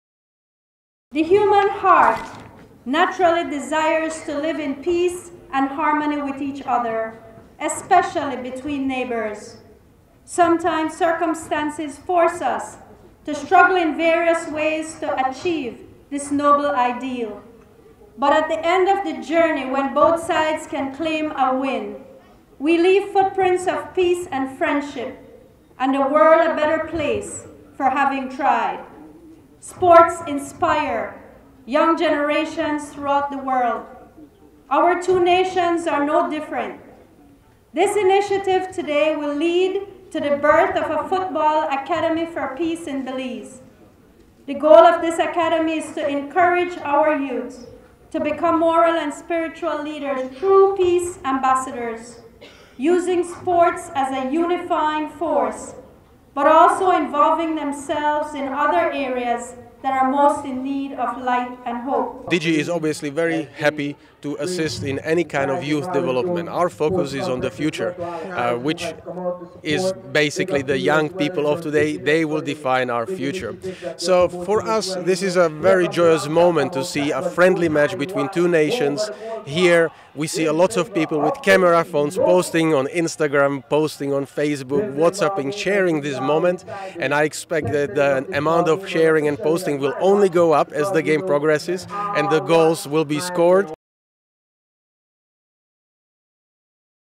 Football 4 Life – an International Friendly between Belize and Guatemala
A first-of-its-kind Football for Life peace match took place today at the Marion Jones Sporting Complex in Belize City. The under-eighteen national teams from Belize and Guatemala converged on the pitch for the international friendly organized by the In the Gap Institute in collaboration with Belize Telemedia Limited. While the mission is to promote sports diplomacy from a young age, Maria Zabaneh says that the end goal is to also establish a football academy for peace in Belize.